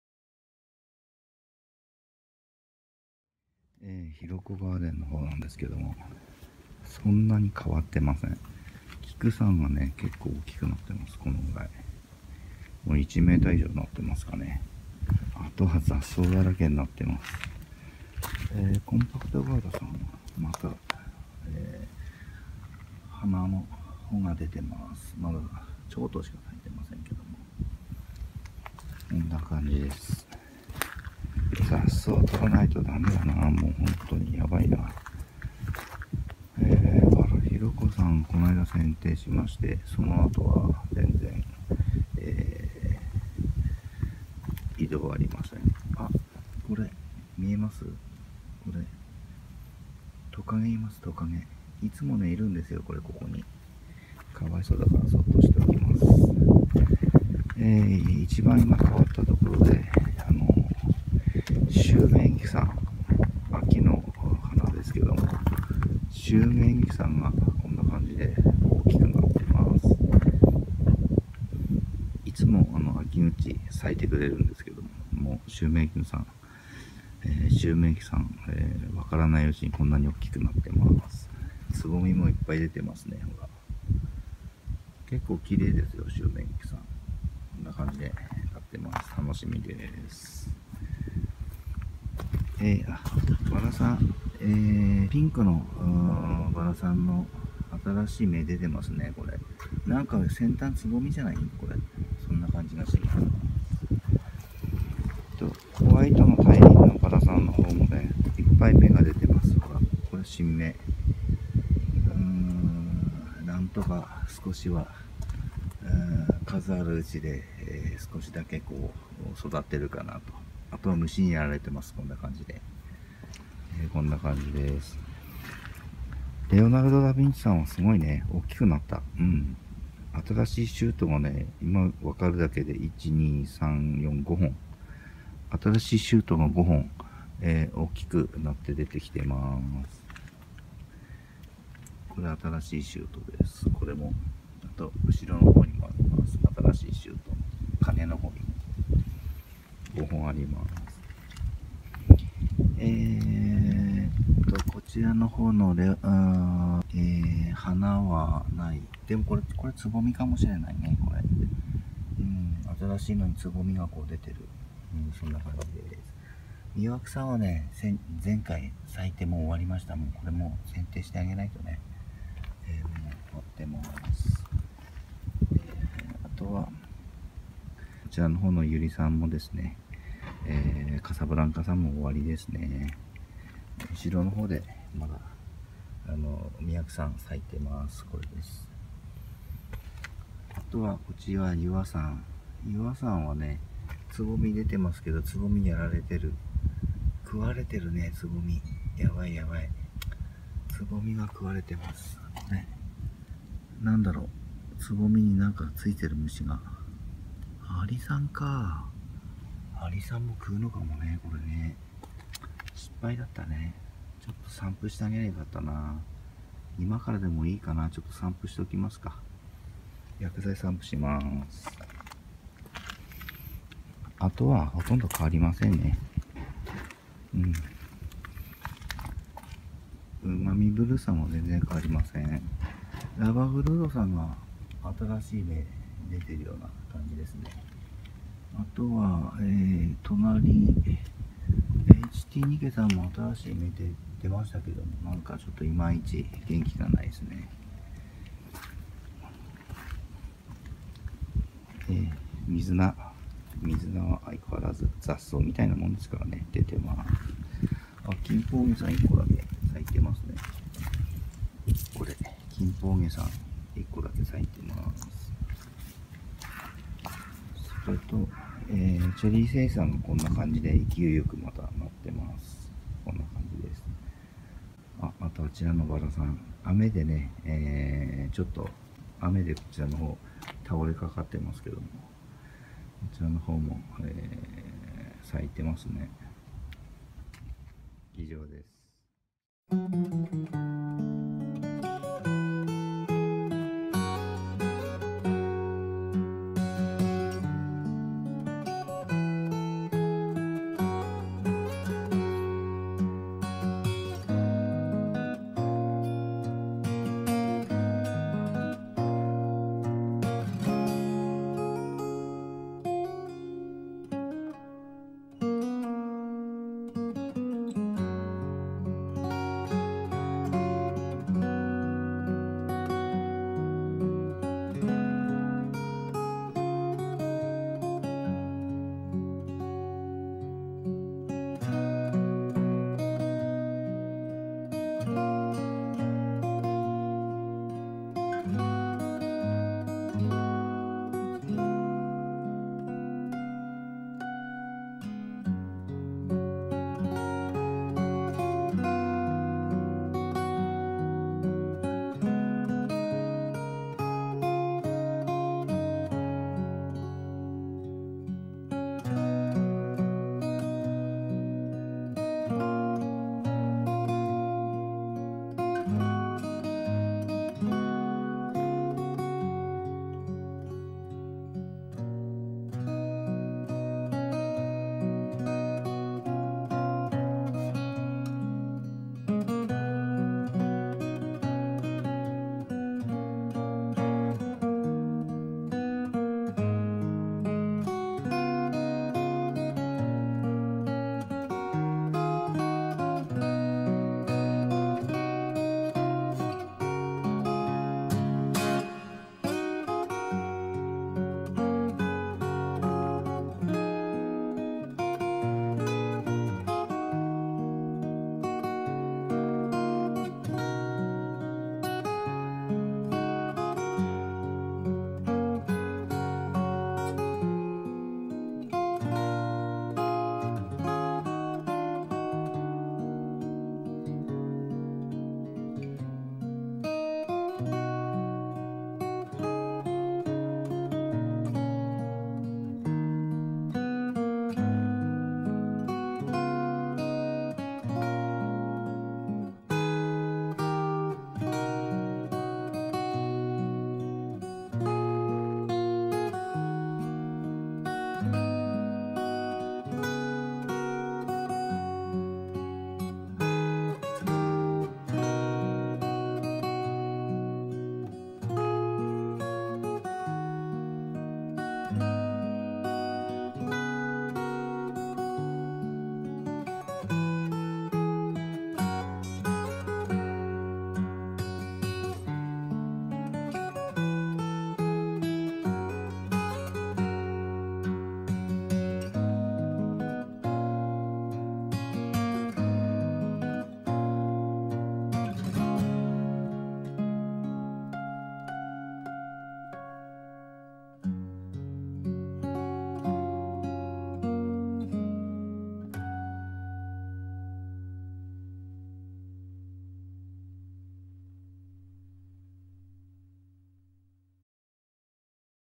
Flower191 妻の残した…🌻ヒロコ・ガーデンの「薔薇・秋明菊とその他花々達」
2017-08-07 自宅の現在の花々達の様子です。
2015年2月より妻の残した🌸趣味を受け継ぎ每日自問自答を繰り返しながら見守り育てております。今年で3年目に突入いたしますが、毎日いろいろな事があり初心者の私には日々勉強です。
花さん達は一週間と短い期間に素晴らしい変化を生み私の予想を上回る光景を見せてくれています。それがガーデニングの虜にする由縁かもしれませんね…薔薇のシーズンには今まで数々の薔薇さんより綺麗な香りのある贈り物をいただきました。また何より私にとっては皆さんに観て頂き尚一層の励みになっております。とても感謝致しております。
=本日のおしながき=
01.寒菊さん達の様子
02.コンパクトガウラさんの様子
03.薄いピンクの薔薇ヒロコさんの剪定後の様子
04.ワンちゃんの置物に・・・
05.秋の花「秋明菊」さんが蕾を出し今にも咲きそうになってます
06.ピンクの大輪強香の薔薇さん新芽を出しましたが・・・
07.ホワイトの大輪強香の薔薇さんの新芽がたくさん出てますがすでに虫にやられているものもありました
08.ツル薔薇レオナルド・ダ・ビンチさんのシュートがたくさん出てきました
09.薔薇レディーガーデナーさんもチッサイ蕾を付け始めました
10.百合カサブランカさんは終了しました
11.薔薇魅惑さんの花が早くも終了
12.薔薇結愛さんの蕾が虫にやられてました
13.薔薇マミー・ブルーさんに変化無し
14.薔薇ラバグルードさん花摘み後新芽を出し始めました
15.薔薇ＨＴニケさんの新芽が出始めました
16.金鳳花さんがたった一個花壇の隅で咲いてました